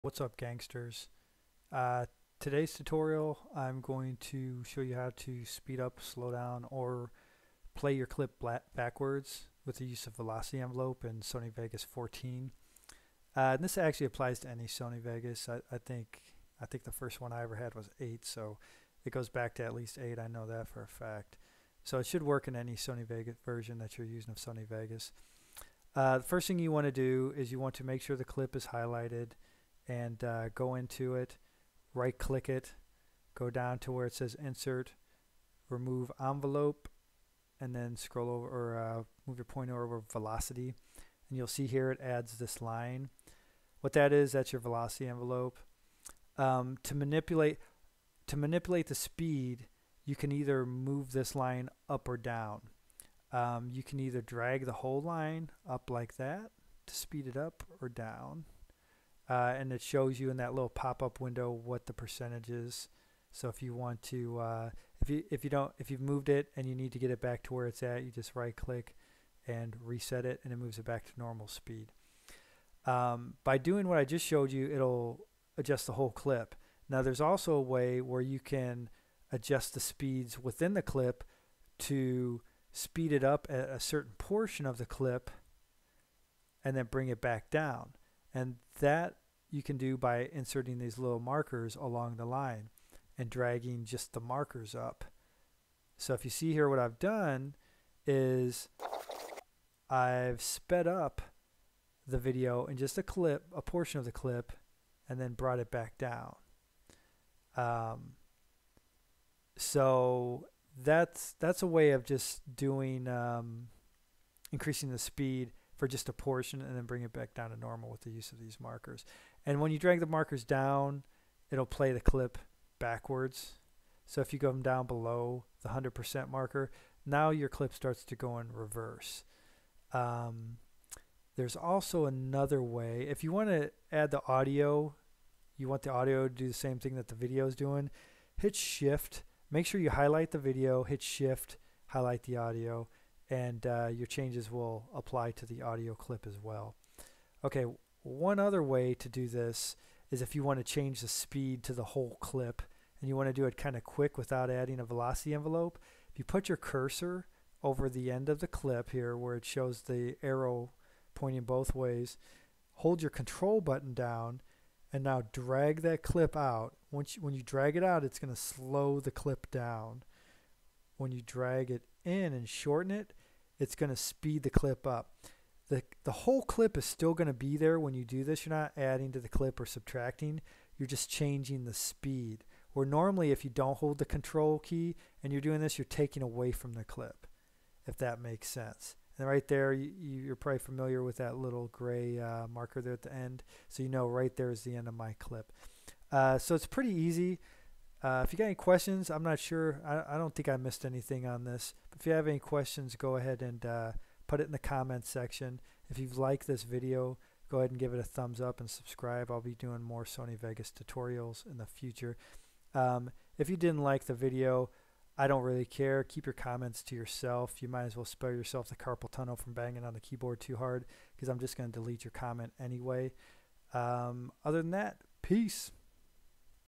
Sony Vegas Pro 14 Tutorial: How To Speed Up Slow Down Or Play Video Backwards
In this tutorial, I will show you How To Speed Up Slow Down Or Play Video Backwards in Sony Vegas 14 Pro.

for cashback on all your online purchases at stores like Amazon, Walmart, Target, etc...